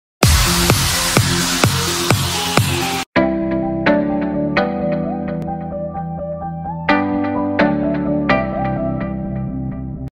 PREMIER LEAGUE PREDICTIONS | Match Week 19
Hello Everyone

Welcome back to another video today’s video is another predictions video and this video is were I have to try and predict the fixtures for the upcoming league.

So if you do enjoy this video be sure to leave this video a thumbs up 👍

And for more amazing content coming soon

This weekend the biggest clubs in action:  Liverpool, Chelsea and Man City

But until next time

Peace ✌️